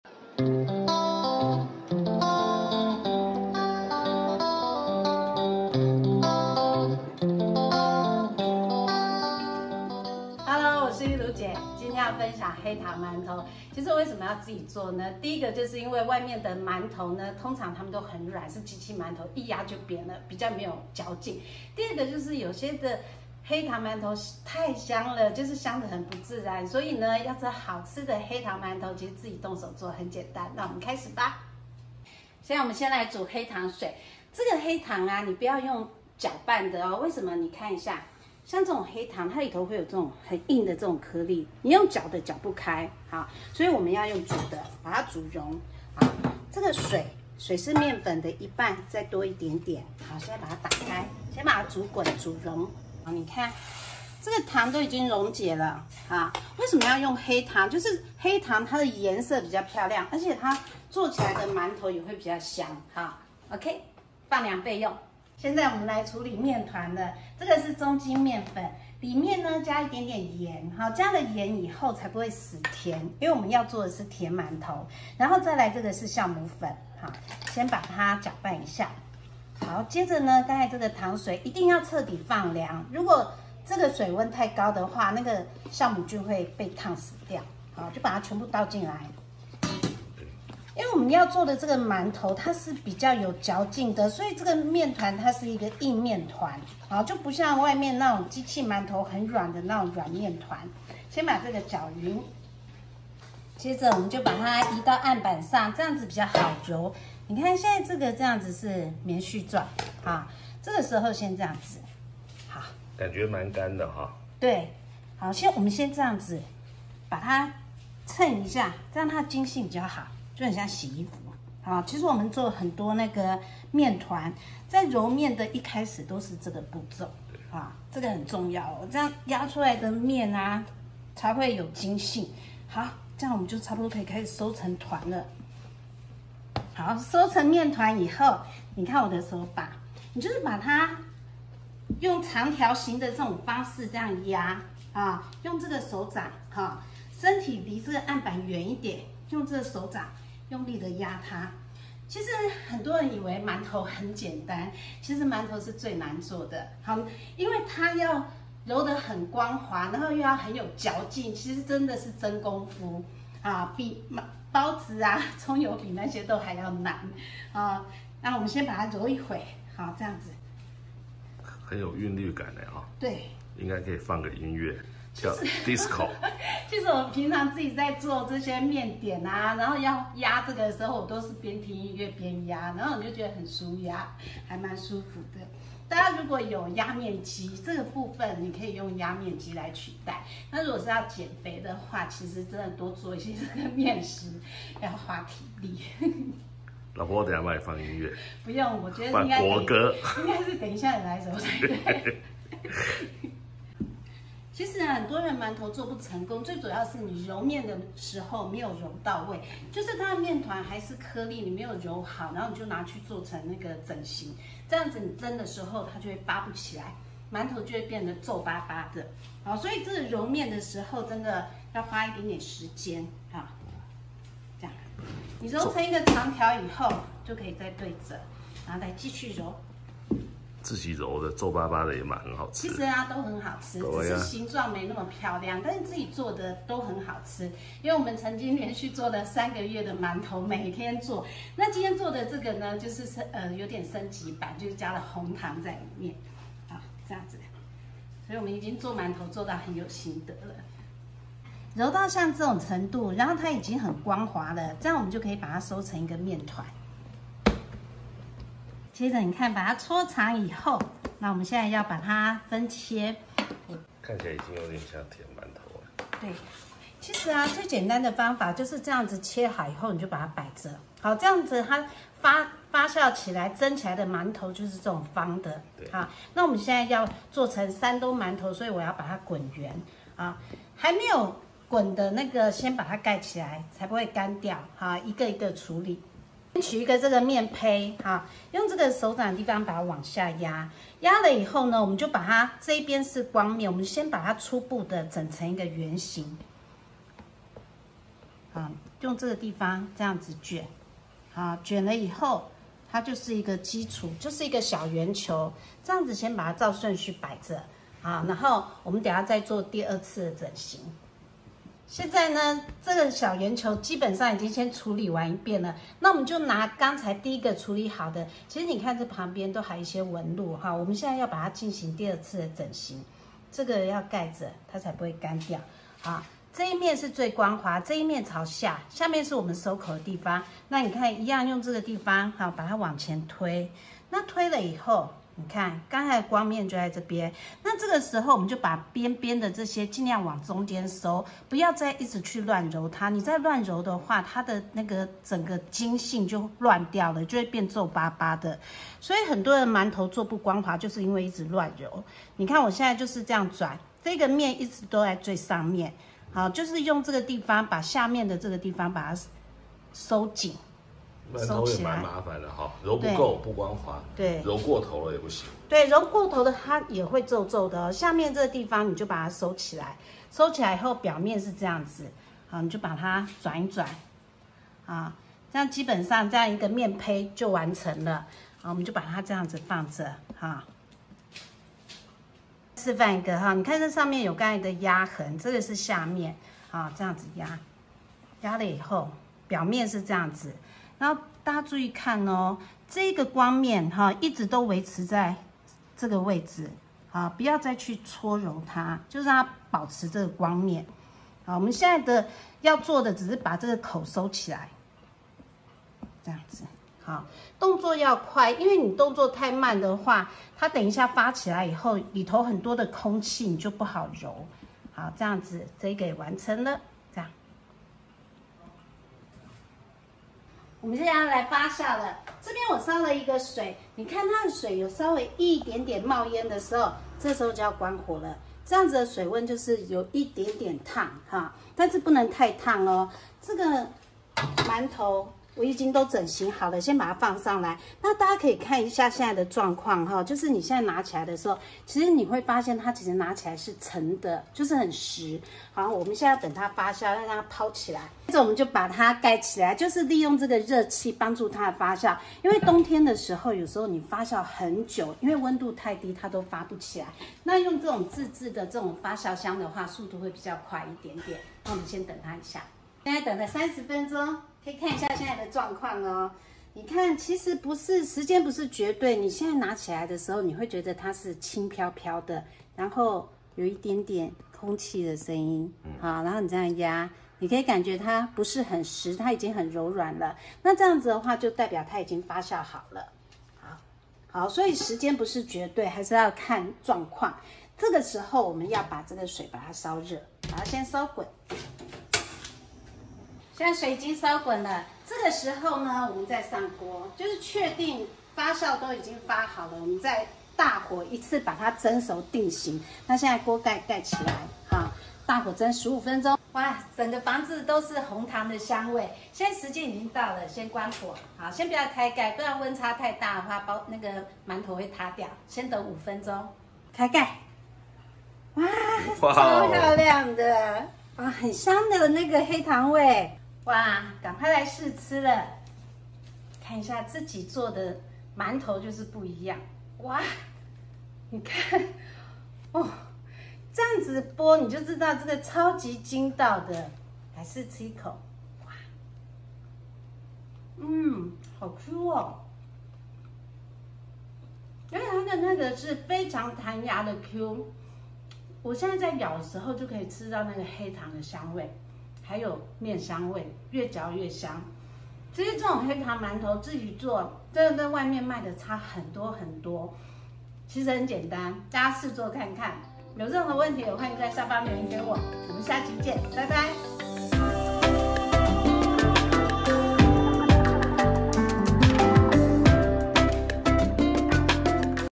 黑糖饅頭、有嚼勁的山東饅頭、揉麵是關鍵、黑糖香+麵香Sweet buns
中筋麵粉 300g
all purpose flour
酵母粉yeast 3g
鹽 salt 1g
黑糖 brown sugar 75g
水 water 160g

🟢黑糖水放涼
Put the brown sugar water until completely cool
🟢分切成7個，每個約70克
Cut into 7 pieces, about 70g each
🟢發酵至1.5倍大，滾水大火蒸15分鐘，熄火、五分鐘後再開蓋
Ferment until 1.5 times larger, steam in boiling water for 15 minutes, turn off the heat, and open the lid after five minutes